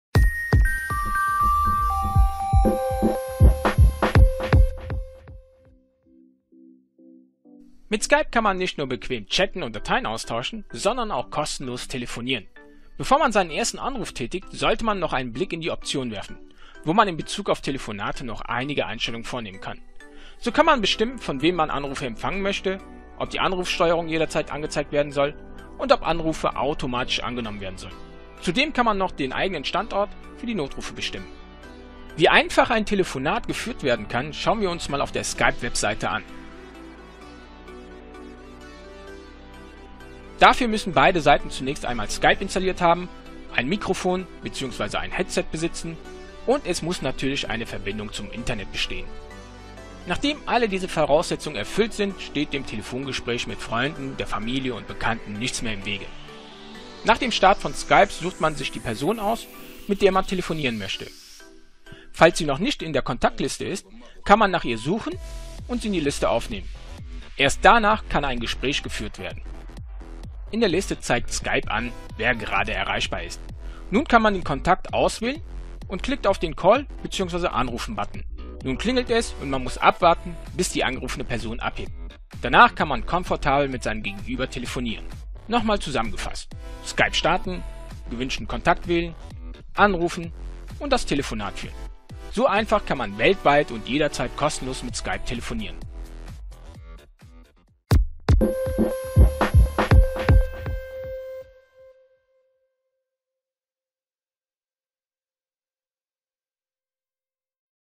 Skype - Weltweit und kostenlos Gespräche führen | Winload.de
Skype-Nutzer können untereinander, egal wo sich der Gesprächspartner auf der Welt befindet, kostenlose Gespräche führen, chatten und Dateien austauschen.